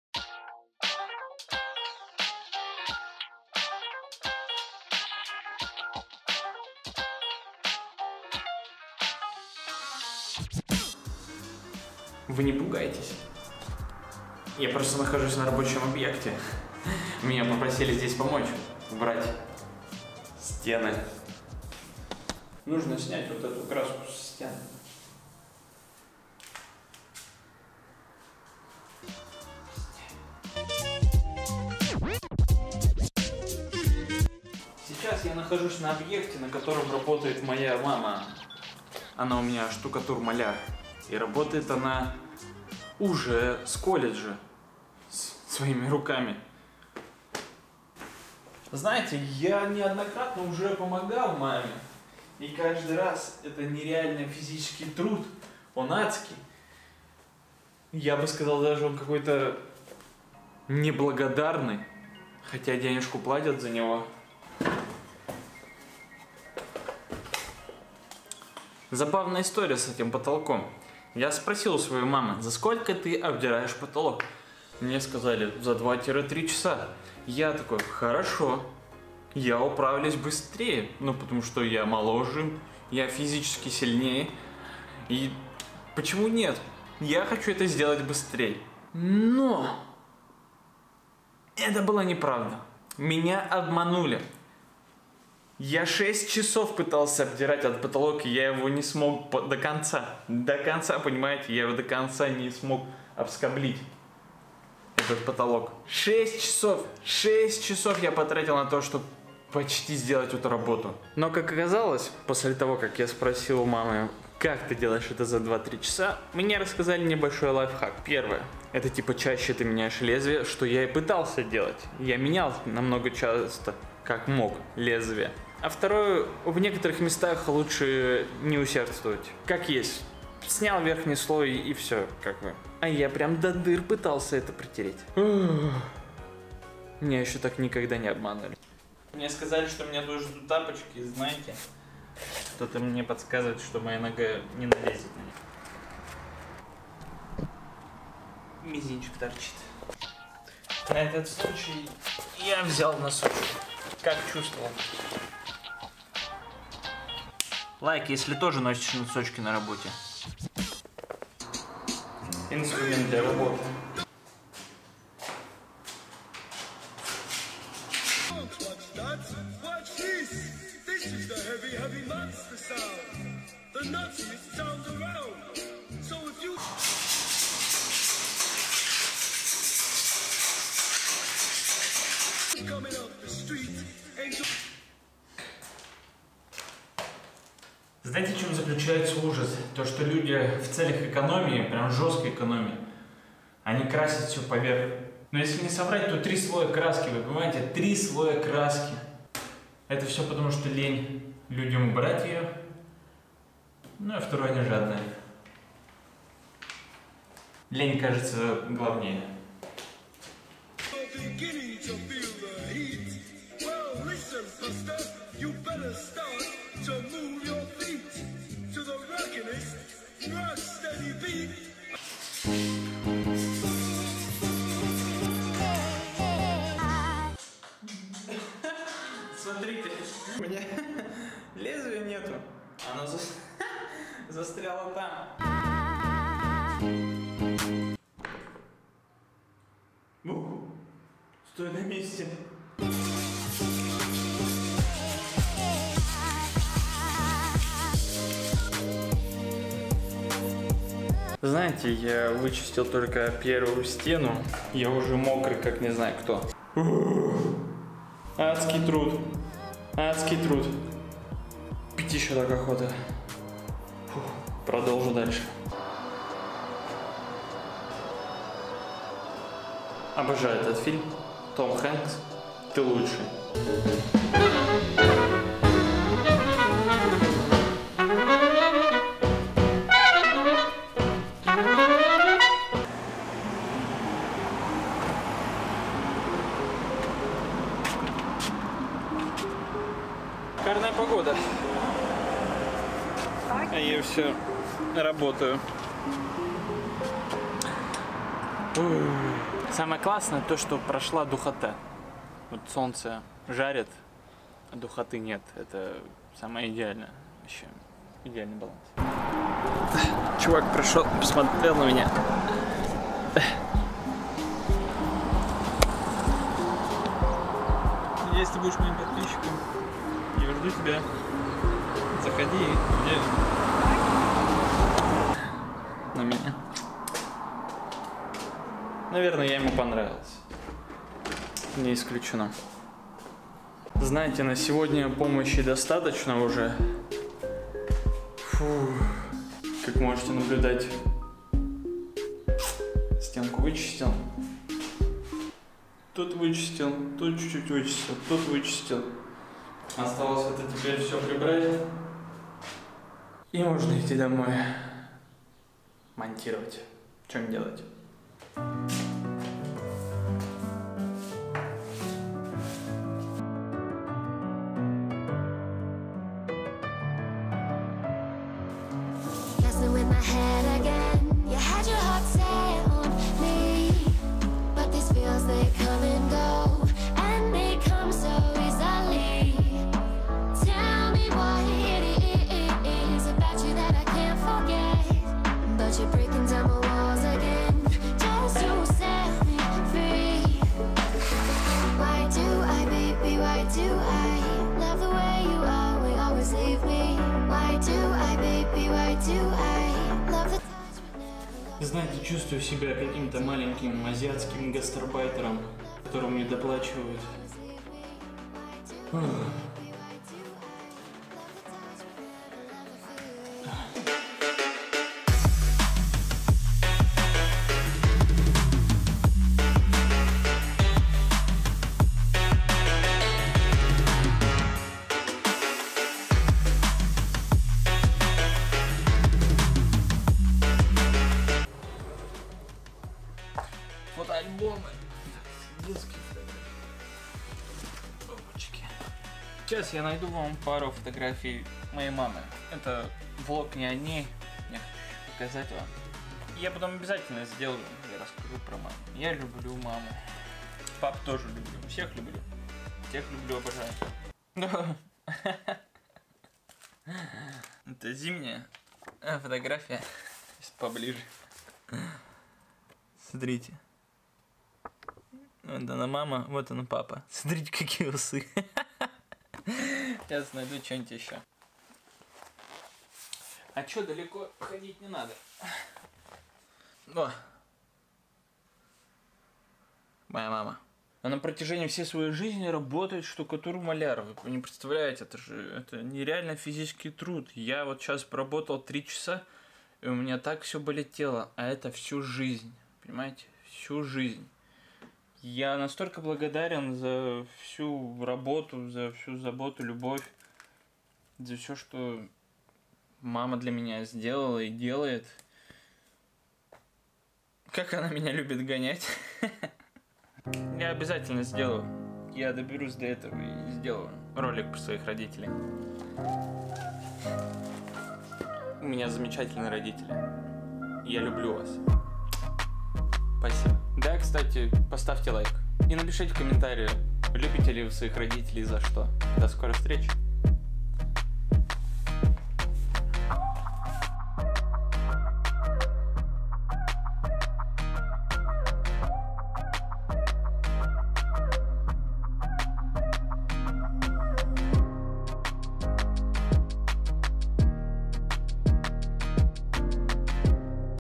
Работник месяца
Тяжелые будни блогера, когда денежки нет, а кушать хочется. Вот чем зарабатывает новый блогер, пока не стал успешным, как Николай Соболев. Шутка, все круто ;)